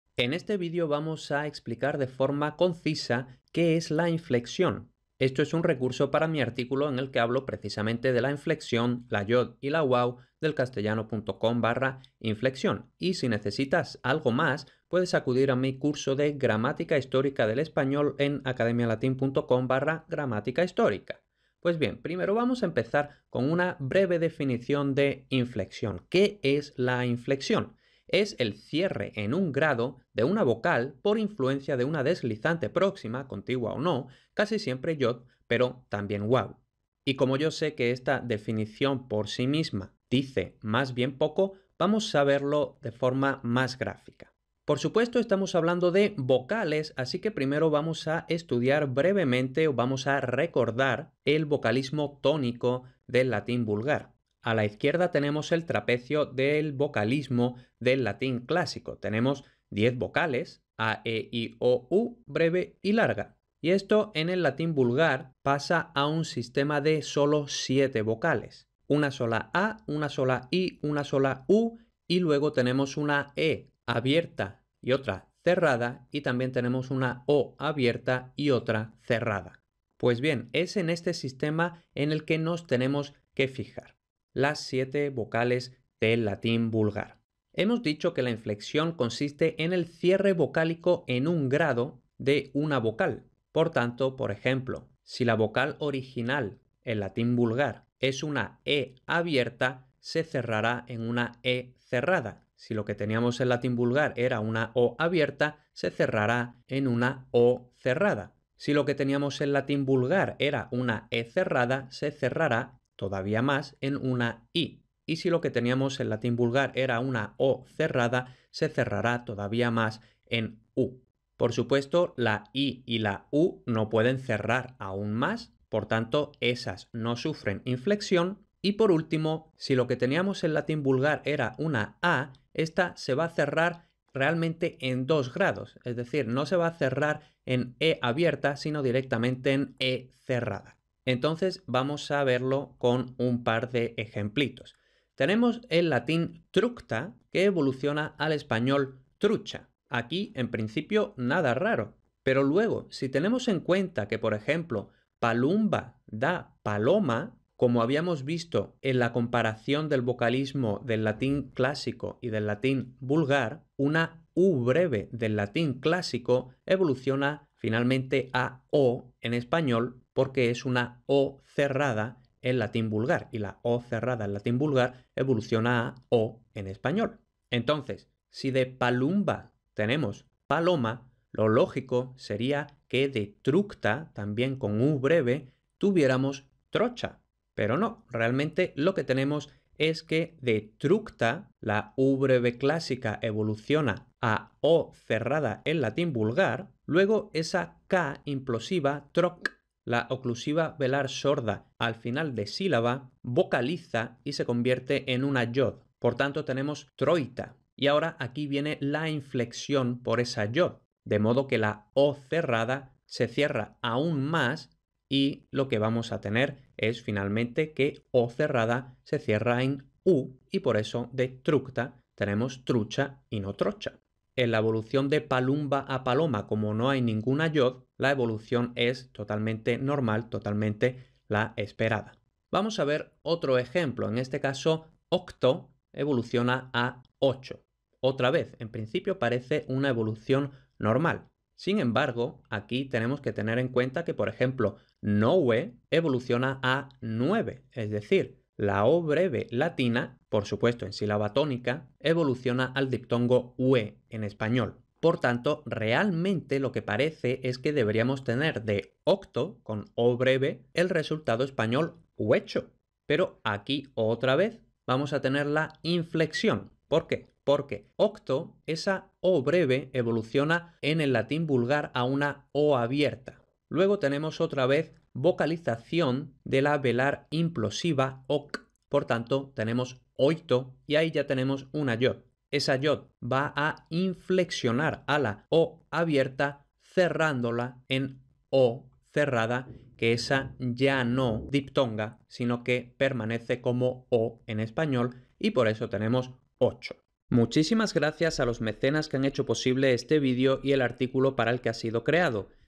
QUÉ ES LA INFLEXIÓN: definición y explicación con ejemplos
La DEFINICIÓN de «INFLEXIÓN» es bien simple. La inflexión es el cierre en un grado de una vocal por influencia de una deslizante próxima (contigua o no), casi siempre yod, pero también wau. (Por tanto, es un tipo de metafonía, una asimilación incluso, aunque este es uno de esos detalles en los que no deberíamos perdernos). Así de fácil, así de simple.

📝 Este vídeo fue creado para acompañar el artículo «Por qué bebemos leche y no latte: la inflexión, la yod y la wau»